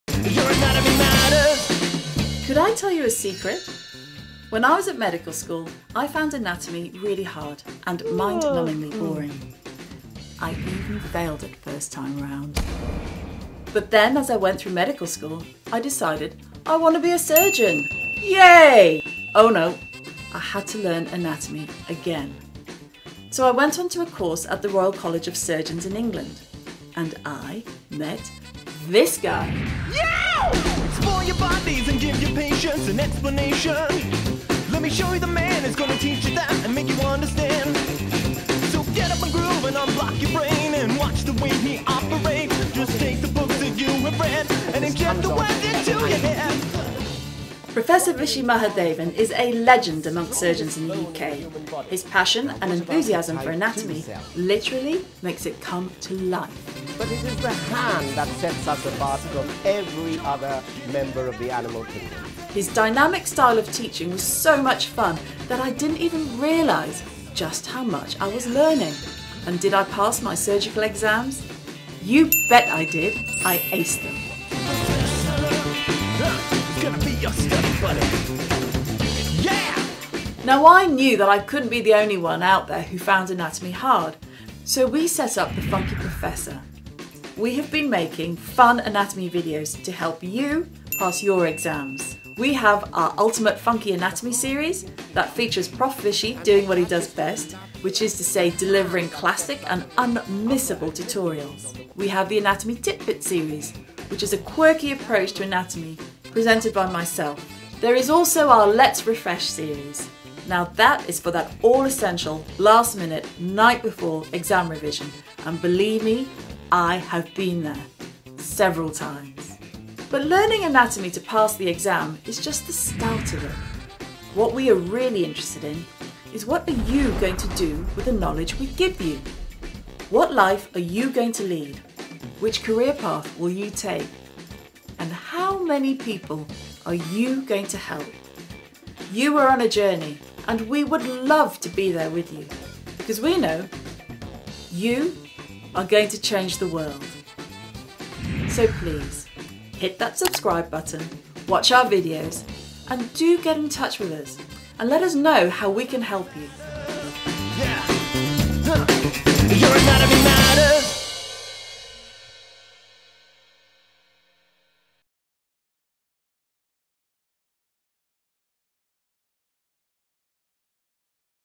The Funky Professor - Inspiring your Human Anatomy Journey
Anatomy made Fun  Exams made Easy
This is Anatomy like you've never seen it before.

Do you find anatomy hard?  Mind-numbingly boring? Complicated?
The Funky Professor offers you an exciting alternative to traditional methods of teaching of anatomy.

Join Professor Vishy Mahadevan, Emeritus Professor of Anatomy, Royal College of Surgeons and Dr Susan Alexander, Orthopaedic Surgeon, London, as together they bring anatomy to life!

Fun and easy, The Funky Professor team are here to guide you through one of the most amazing journeys of your life:  the intricate, fluid, ingenious anatomy of the human body.

We have collaborated with Biodigital Human, using their engaging interactive digital human to enhance our explanations.